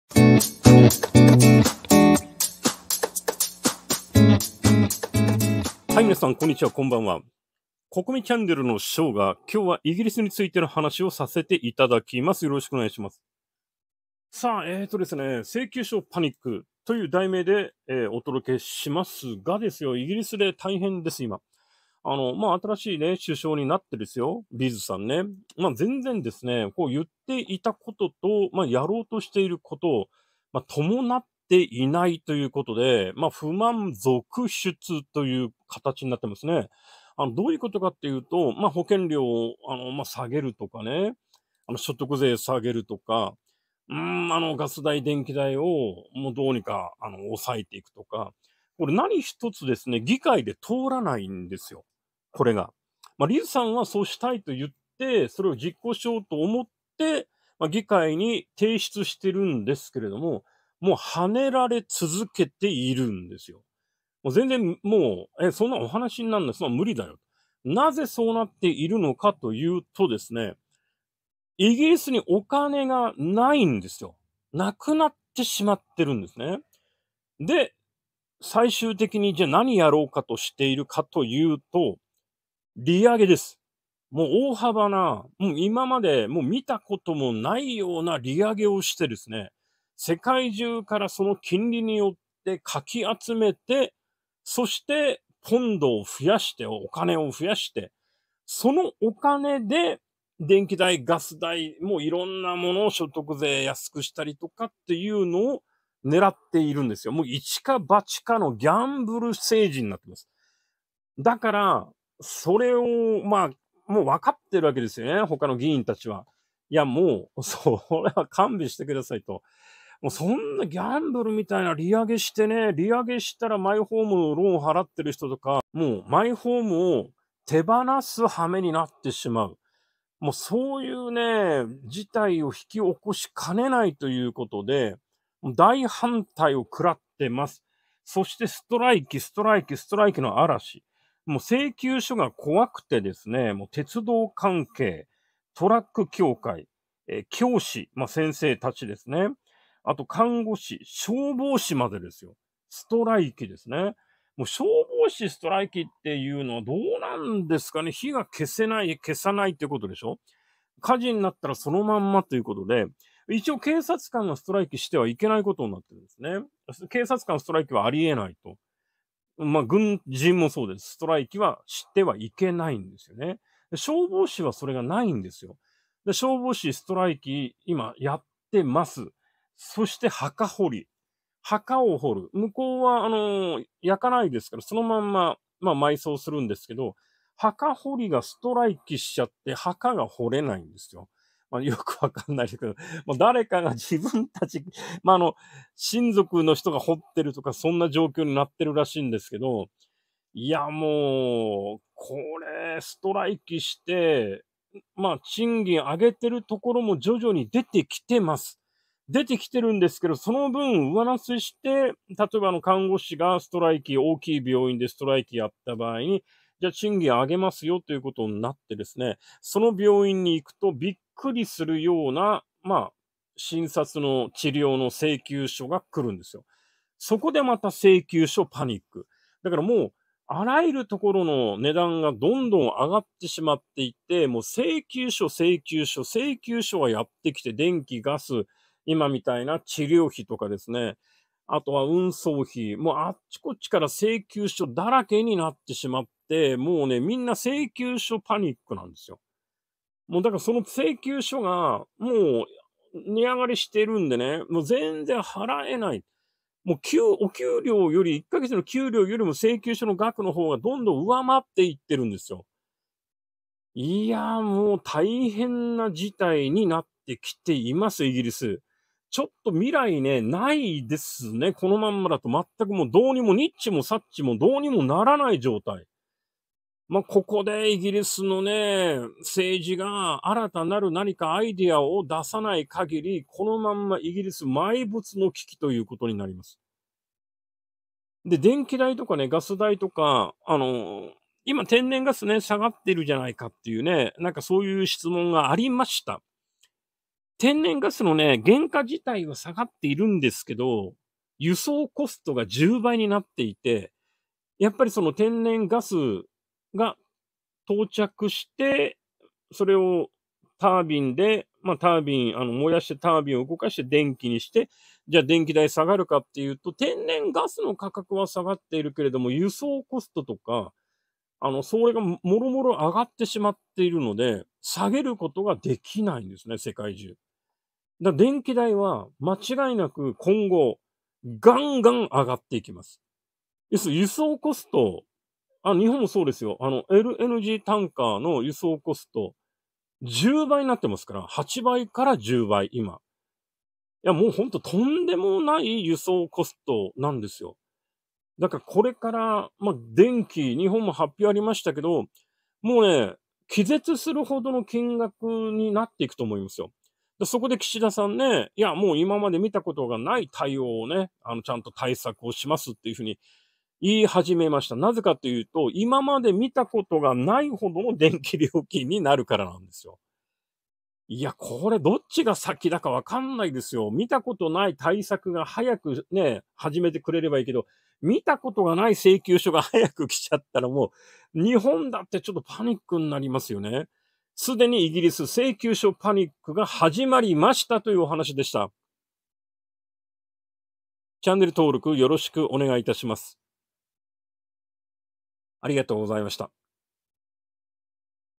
イギリス 請求書パニック 電気料金や ガス料金や医療関係費が急上昇
1970年代にイギリスが急激なインフレに見舞われたが現在その時よりも過酷で厳しいインフレがスタートしている。
電気料金の値上げガス料金の値上がり治療費の上昇運送費のコスト増などによって毎月の支払いが収入を遥かに超えてしまっている人々が続出している。
このまま行くと来年の春までに生活が成り立たなくなる人が人口の30%に達するものと推測される。
まさに危機的な状況にある。